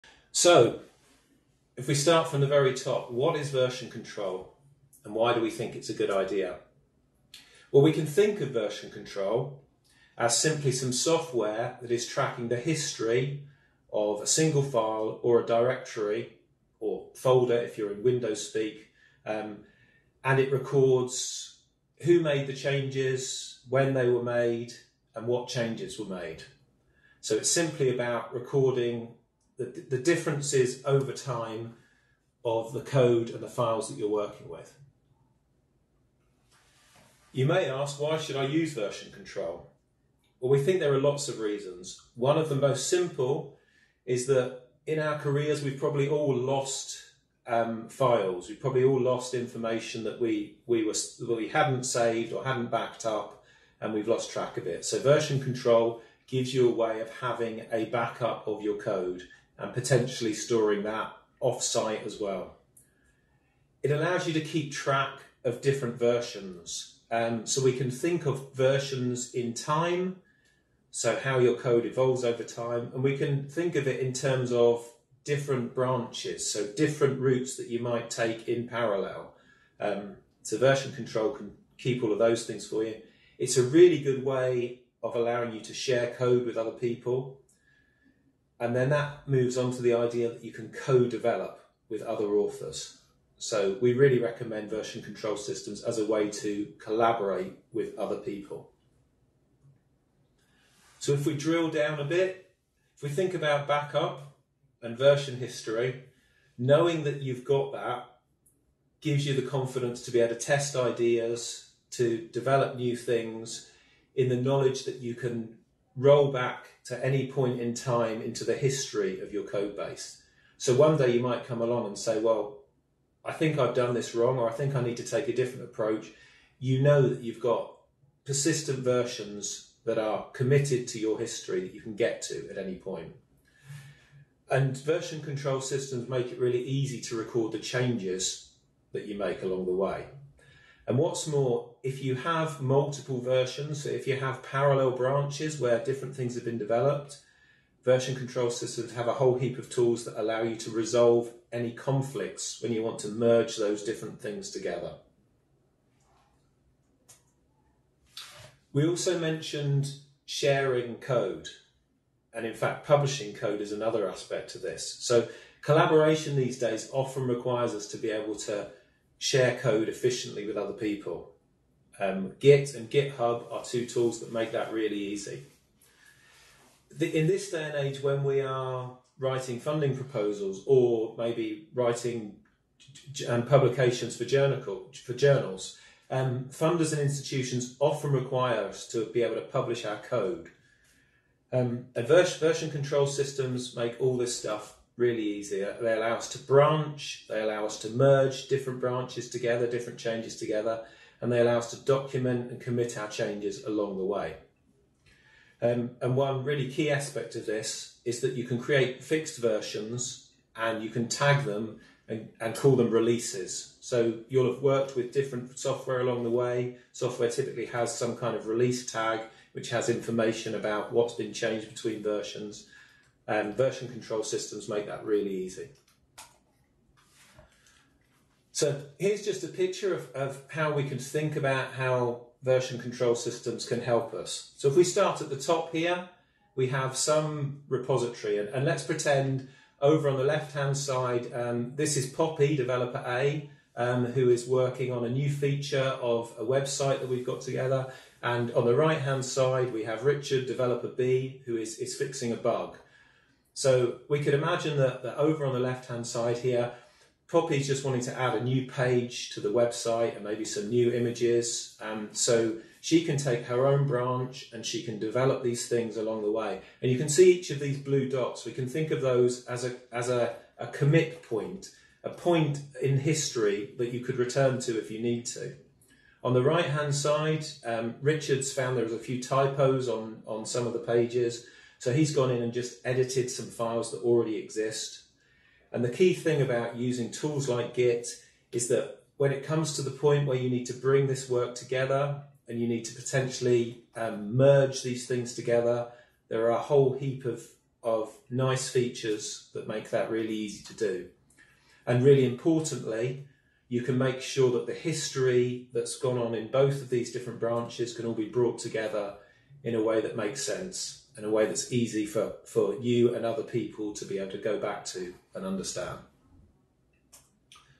Introduction to Git and GitHub Webinar, Section 1 - What is version control and why to use it?
This short clip describes what version control is and why you should use it.

Recorded on 1st November 2018 for the fourth webinar in a series provided by CEDA.